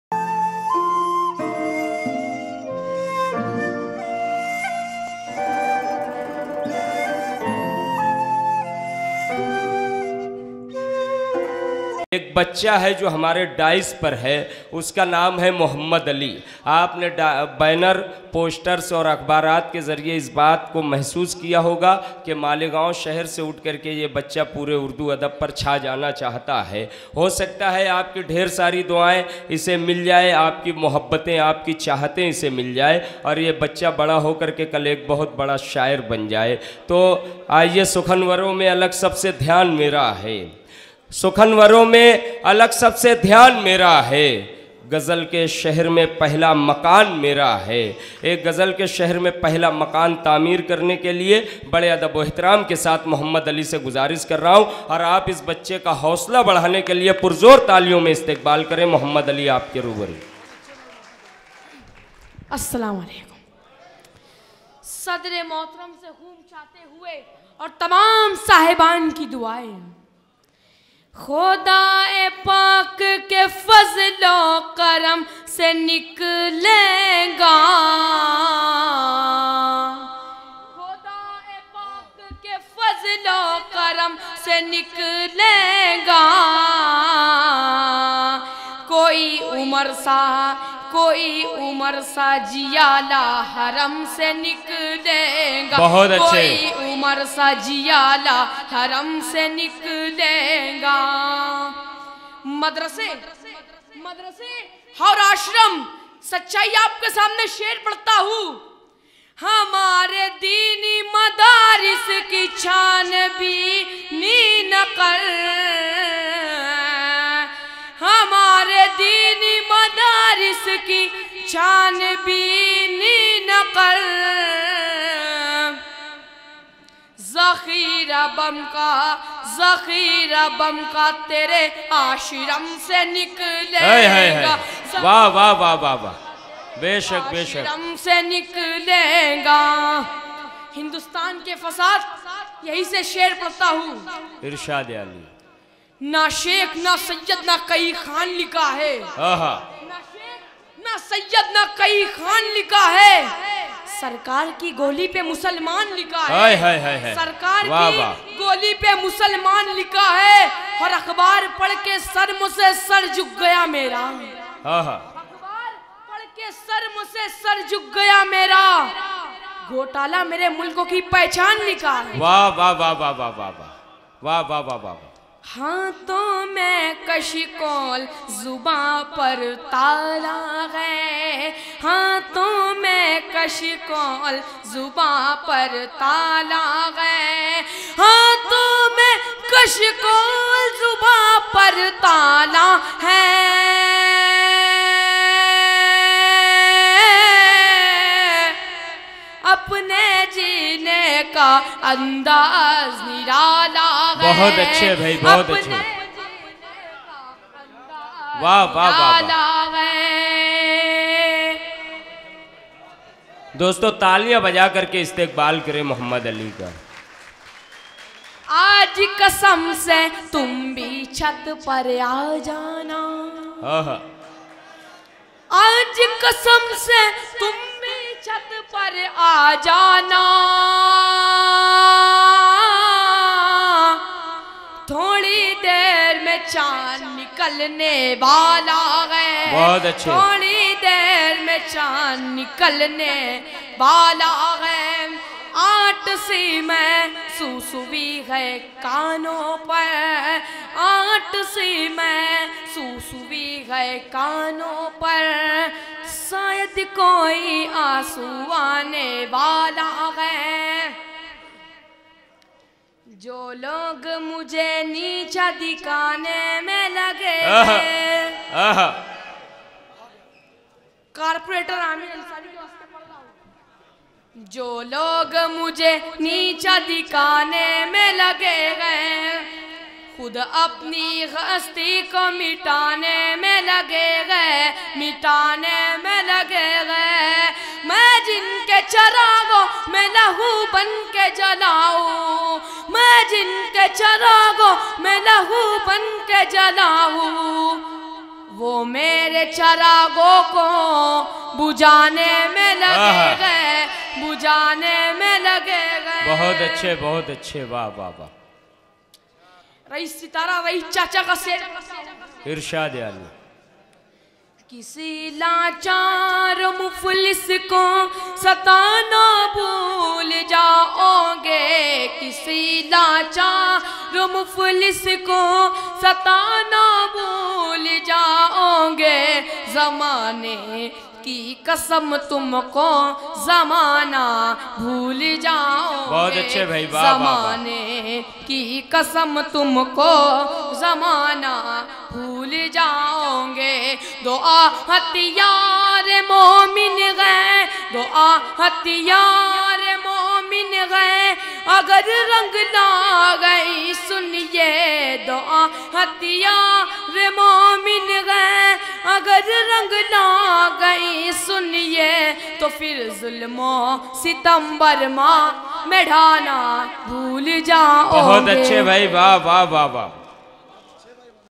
MOHAMMAD ALI,MALEGAON,ALL INDIA MUSHAIRA,ON 15 TH DEC 2017,CON-SUHAIL AZAD.
MOHAMMAD ALI,
EK SHAAM CHANDNI SHABNAM KE NAAM,
ALL INDIA MUSHAIRA AT MALEGAON ON 15TH DEC 2017.
SADARAT-YAHYA ABDUL ZABBAR.
CONVINOR-SABIR HINDUSTANI
                   SUHAIL AZAD.
NIZAMAT-RAEES SITARA